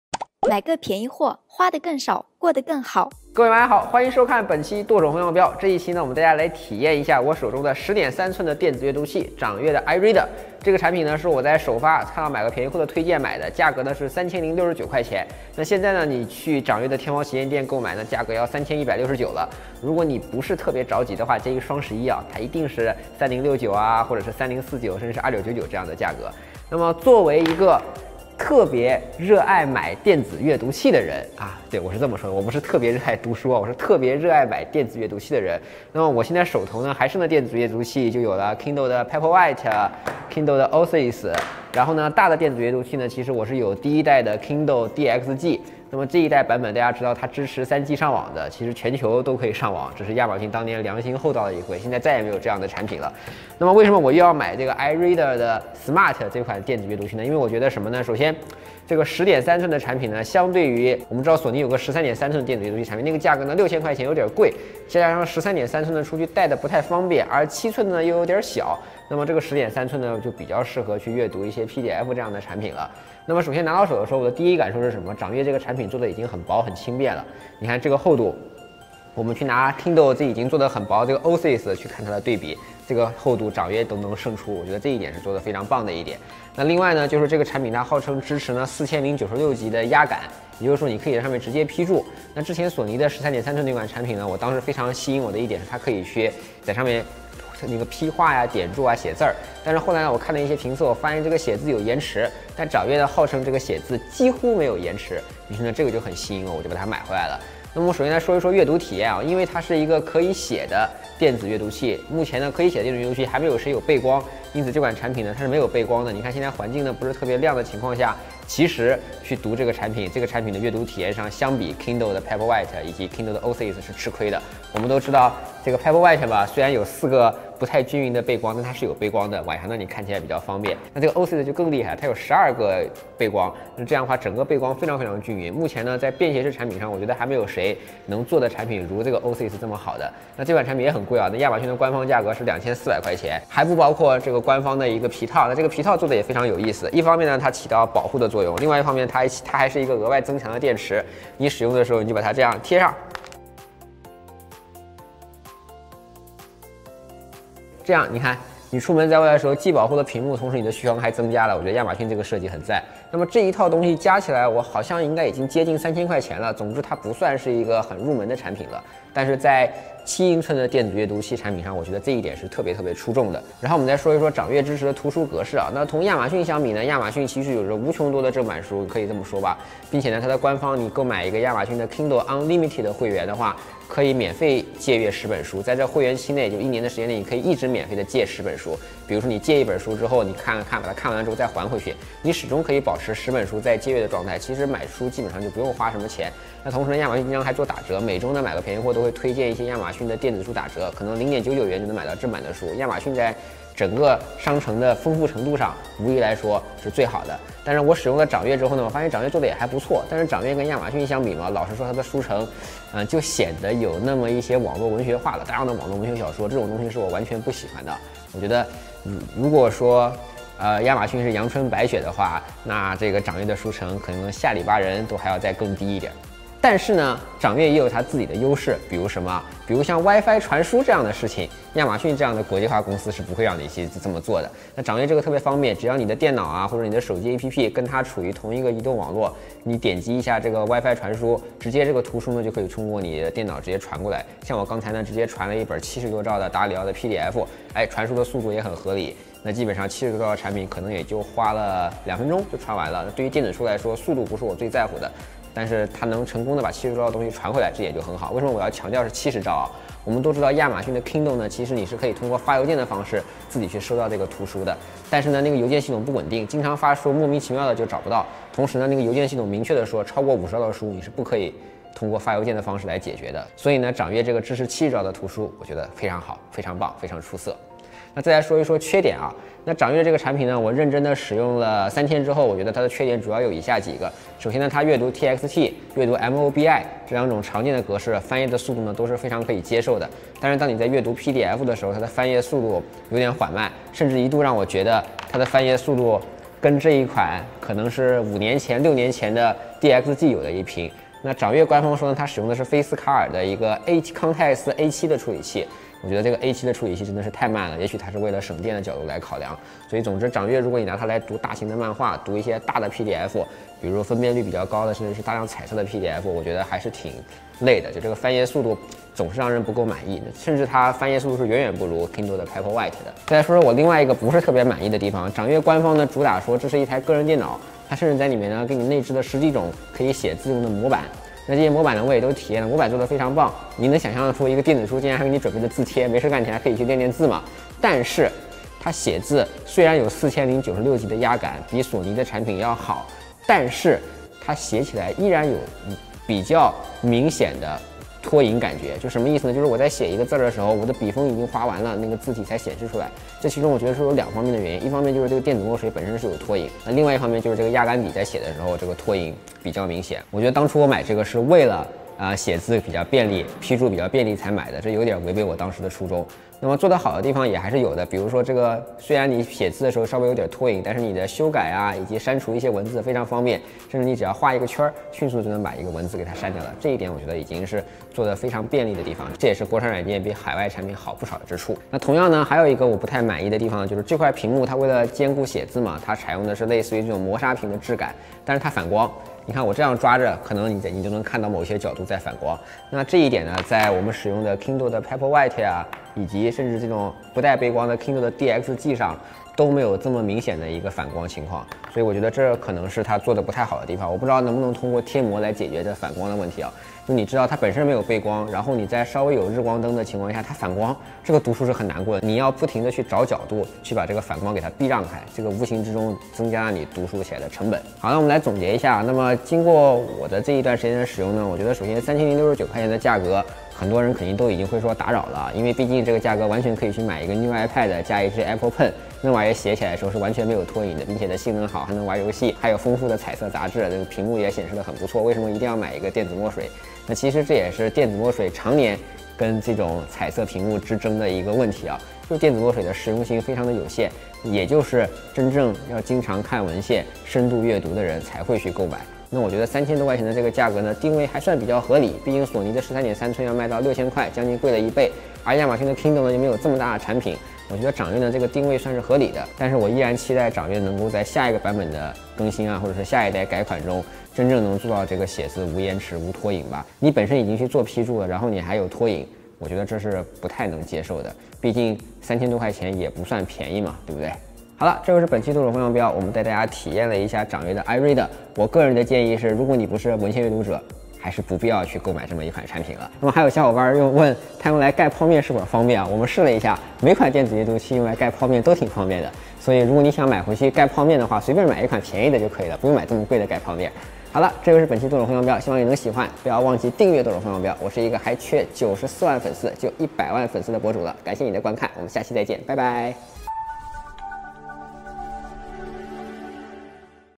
能否打败索尼？掌阅smart可批注10寸电纸书体验
为什么要用电纸书阅读器而不买实体书。微博/微信：剁手风向标，主站：买个便宜货